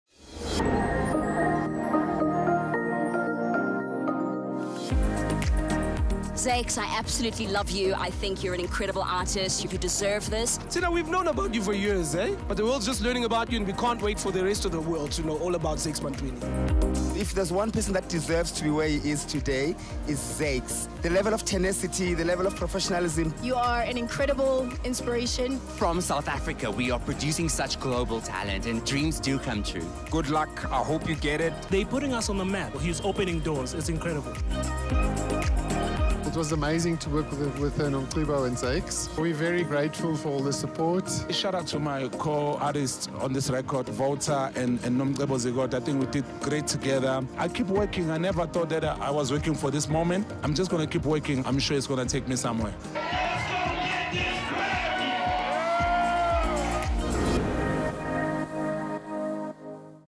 E! Grammy Night Nomination of Zakes Bantwini
Join us on an unforgettable journey as we shine the spotlight on the incredible Grammy nomination and victory of South Africa's very own, Zakes Bantwini! Entertainment and Zioux Bar, brings you an exclusive Events video that captures the essence of this remarkable achievement.

In this Event video, we take you behind the scenes of the electrifying celebration, where Zakes Bantwini's music journey culminated in a historic Grammy win. Witness the joy, passion, and hard work that led to this momentous achievement and feel the euphoria of the music world rejoicing.

🌟 What to Anticipate:
- Experience the excitement and energy of the Grammy celebration at Zioux Bar.
- Meet the talented artists, friends, and collaborators who shared in this momentous win.
- Get inspired by Zakes Bantwini's dedication to his craft and his heartfelt gratitude for his Grammy nomination.

We're proud to have been part of this extraordinary journey with Zakes Bantwini and the music community.